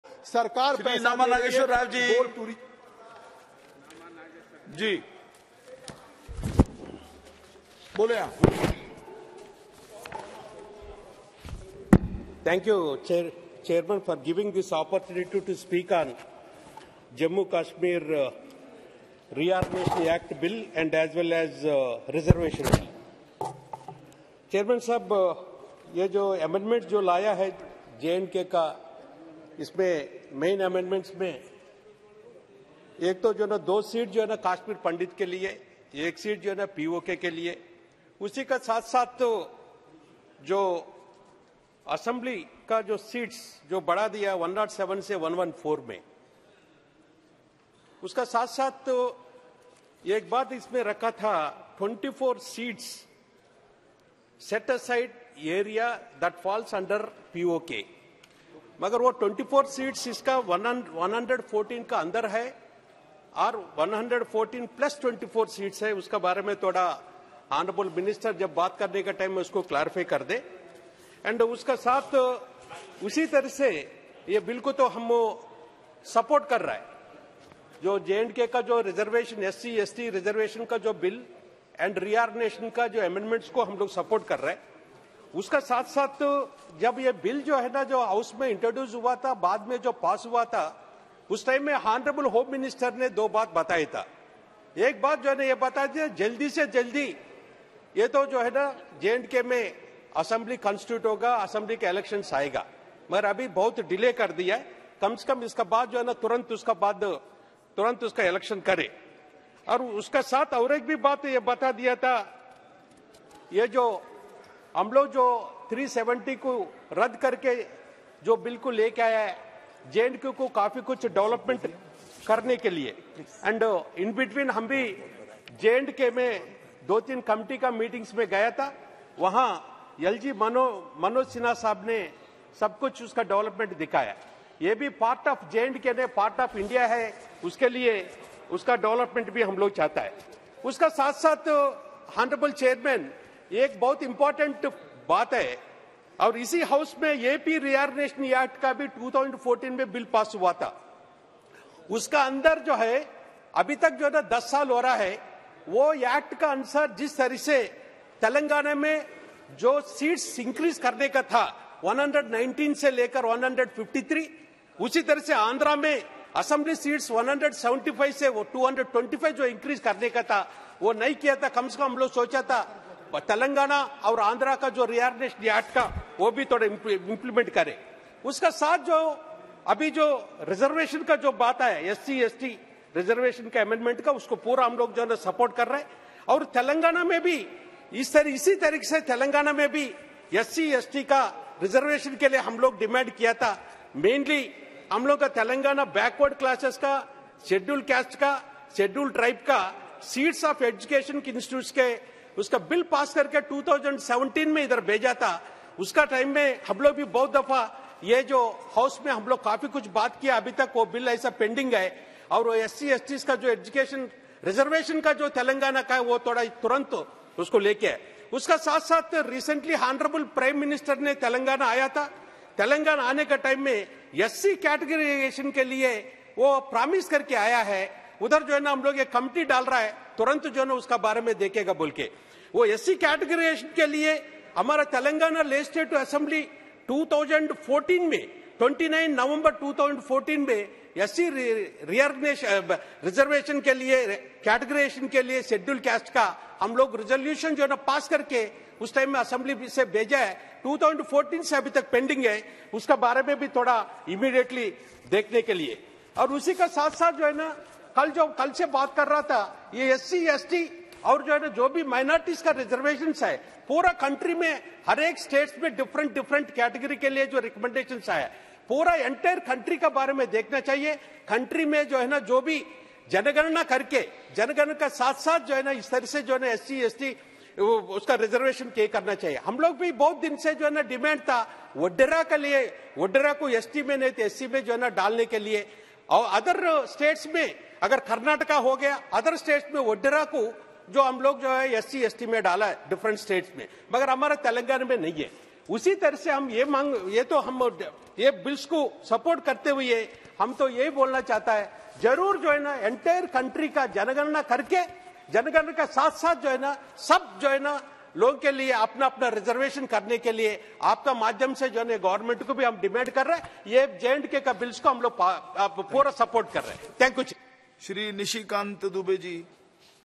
Nama Nageswara Rao's Remarks | The J&K Reservation(Amend)Bill & J&K Reorganisation (Amend)Bill, 2023
Discussion on The Jammu and Kashmir Reservation (Amendment) Bill, 2023 & The Jammu and Kashmir Reorganisation (Amendment) Bill, 2023 in Lok Sabha that  the Bills further to amend the Jammu and Kashmir Reservation Act, 2004 & 2019.